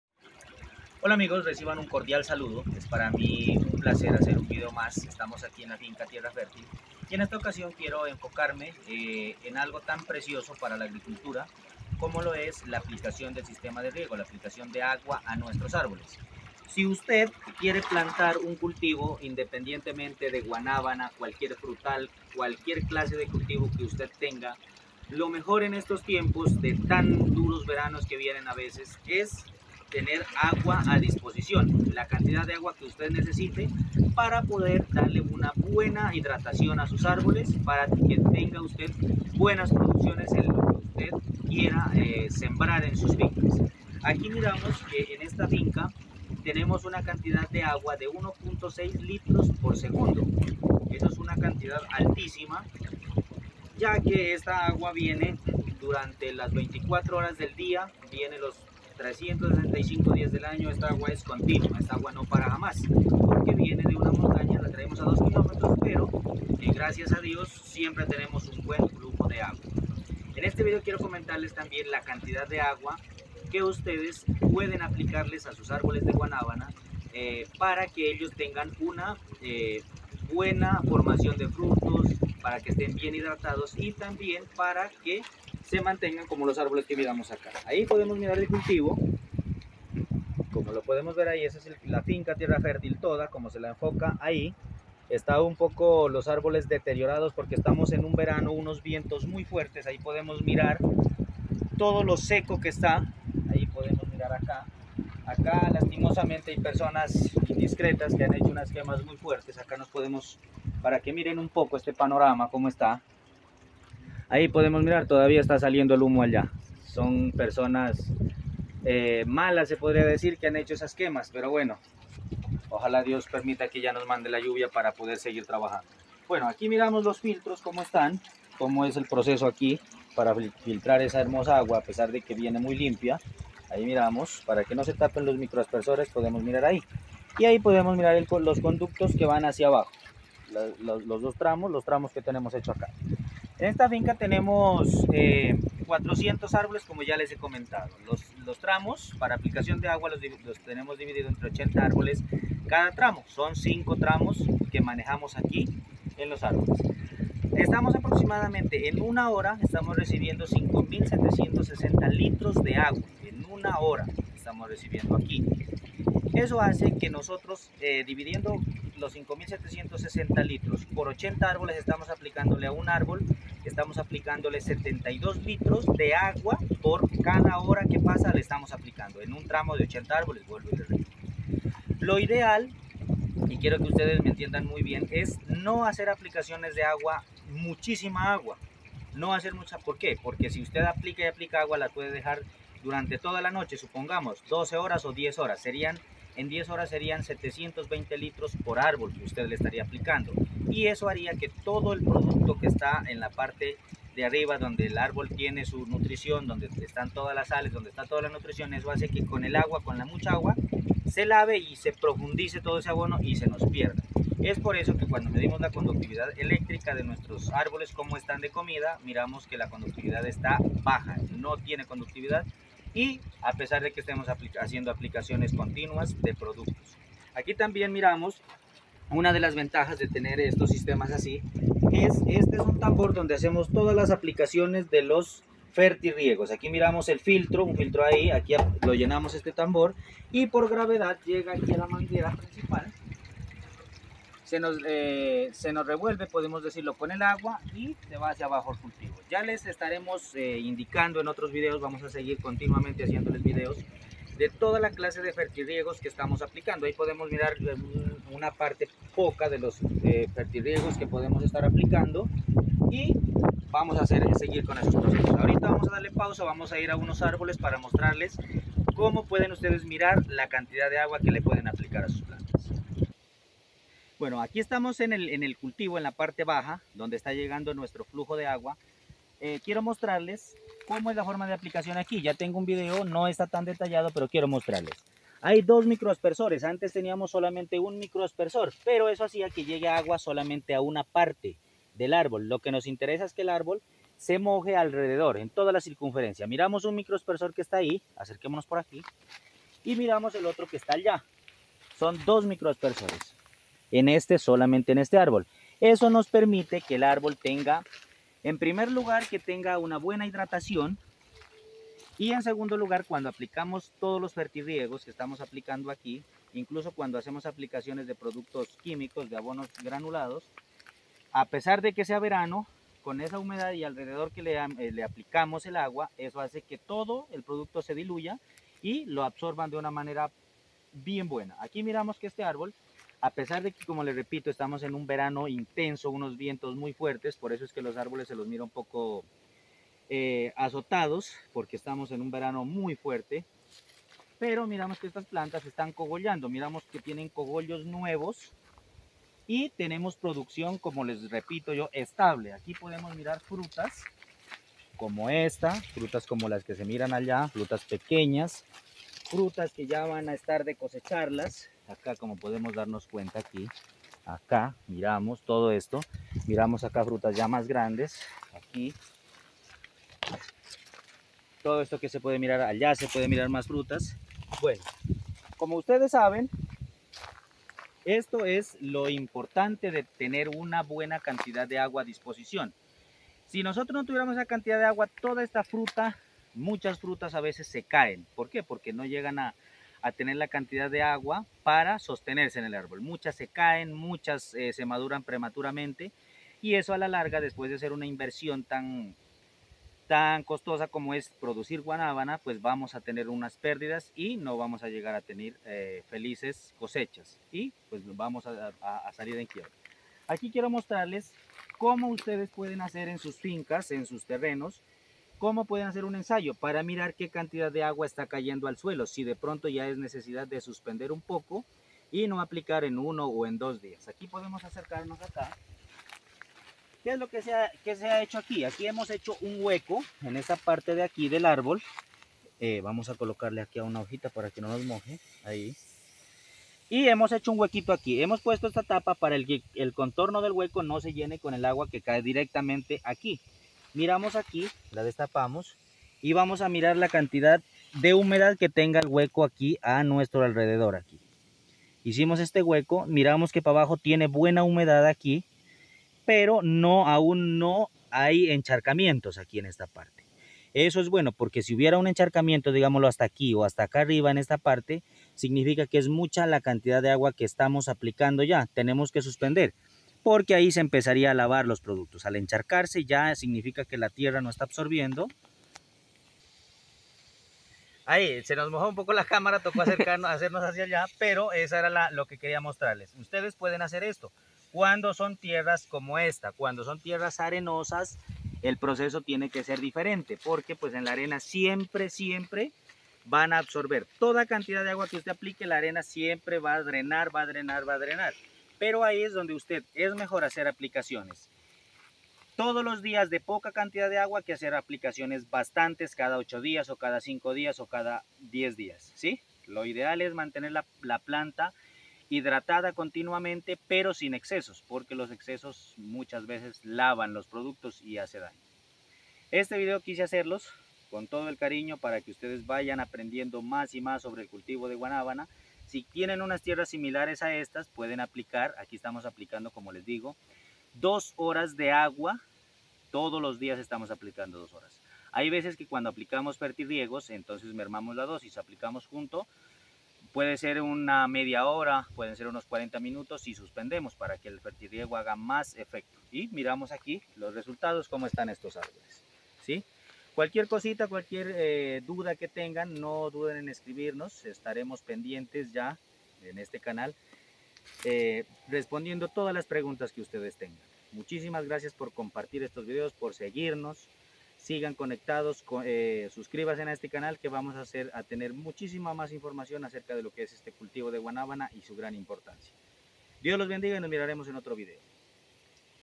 GUANABANA, QUE CANTIDAD DE AGUA APLICAR A LOS ÁRBOLES
En este video, hago conocer un poco las cantidades de agua que se debe aplicar a los árboles de guanabana y la forma más adecuada